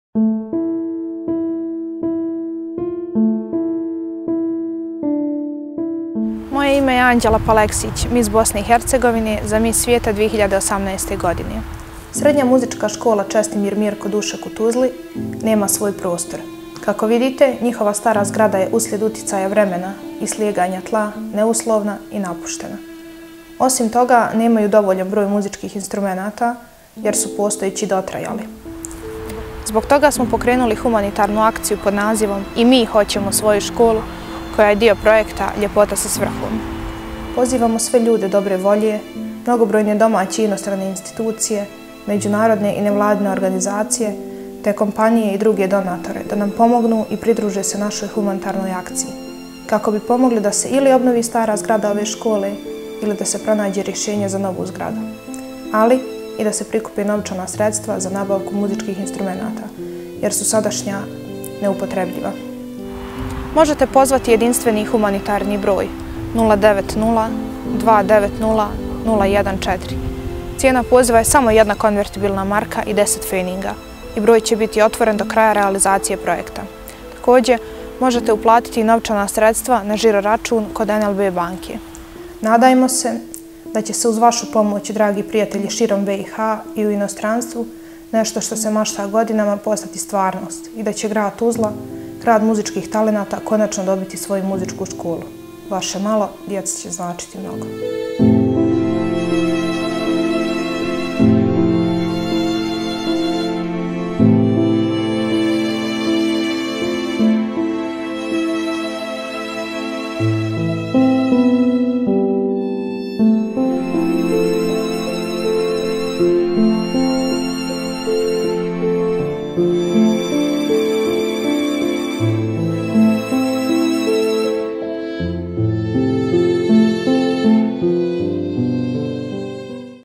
Beauty with purpose - Donation for The Tuzla Music High School
Miss BiH za Miss svijeta 2018 - Anđela Paleksić
Humanitarna akcija prikupljanja sredstava za obnovu srednje muzičke škole u Tuzli i nabavku instrumenta. BUDITE HUMANI I UKLJUČITE SE I VI U OVU AKCIJU!